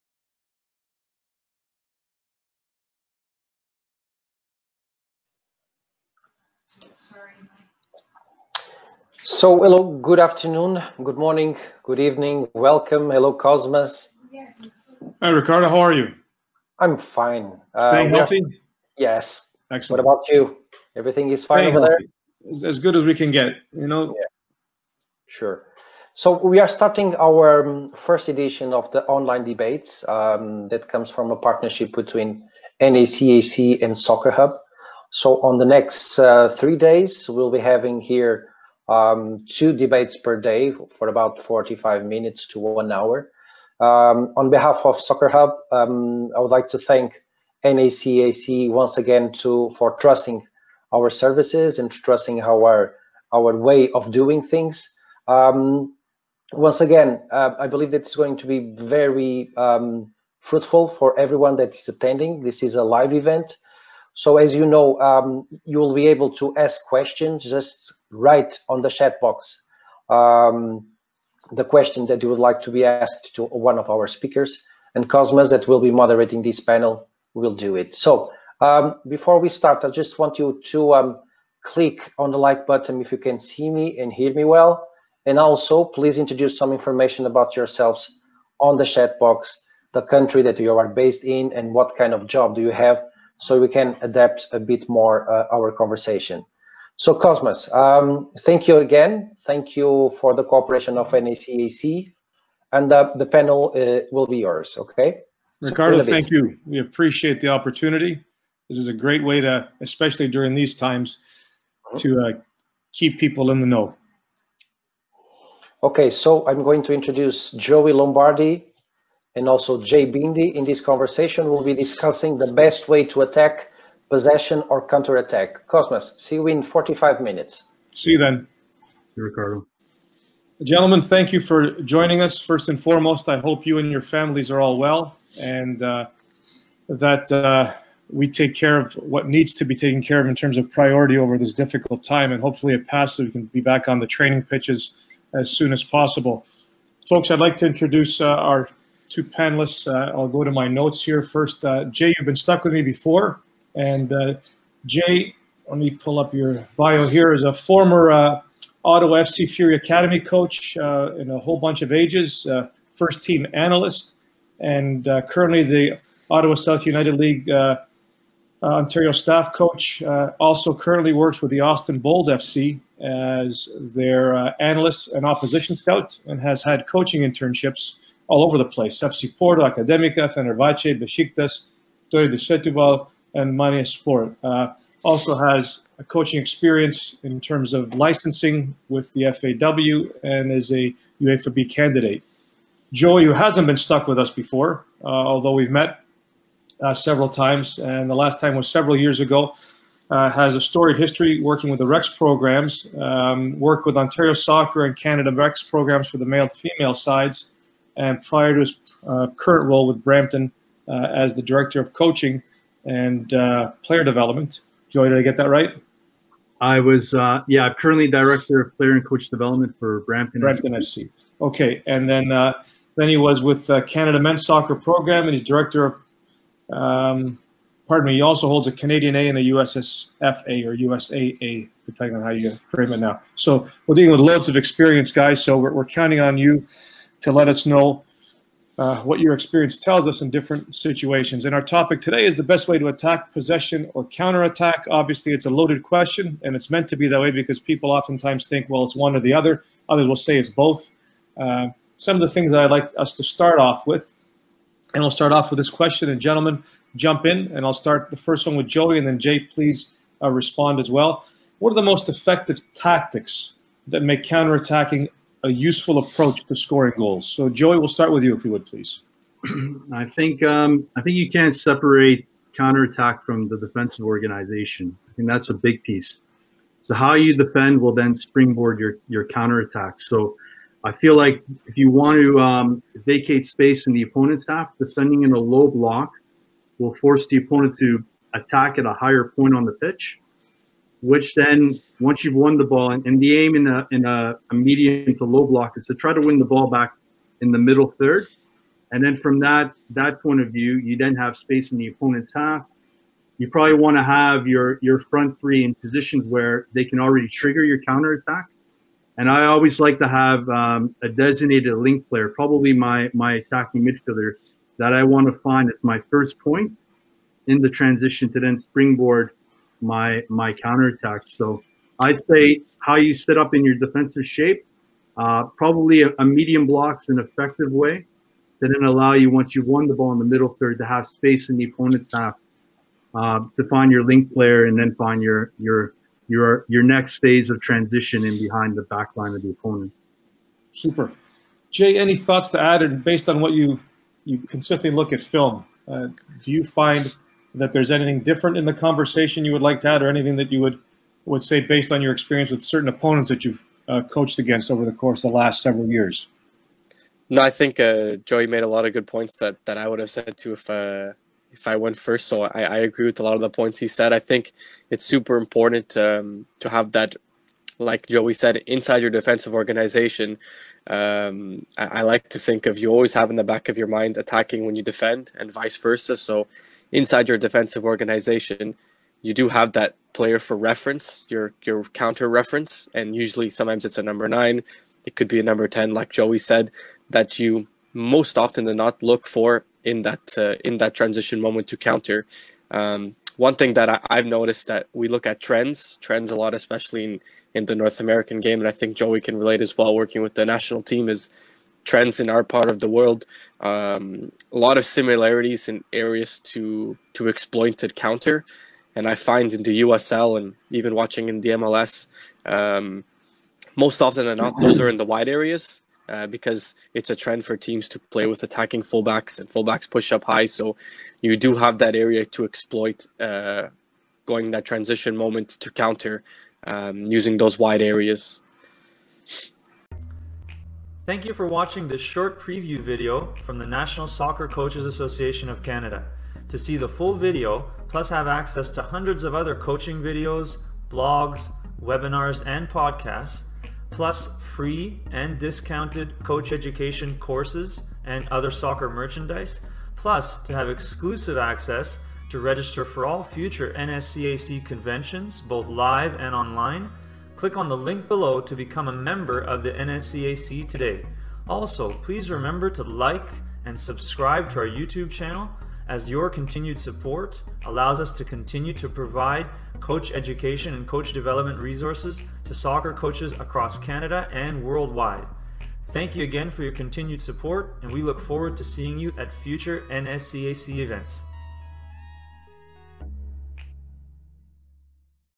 2020 NSCAC Online Debates Panel 1: "Best Way to Attack - Possession or Counter Attack?" (Preview)
Preview of the Expert Panel Debate from the 2020 NSCAC / Soccer HUB Online Debates, titled "Best Way to Attack - Possession or Counter Attack?" featuring expert coaches:

- Kosmas Mouratidis (Director of Coaching, NSCAC)
- Jay Bhindi (Performance Analyst, Austin Bold Football Club)
- Joey Lombardi (Director of Player & Coach Development at Brampton SC)

To see ALL 6 Expert Panel Discussions from the 2020 NSCAC / Soccer HUB Online Debates and MORE, become an NSCAC Member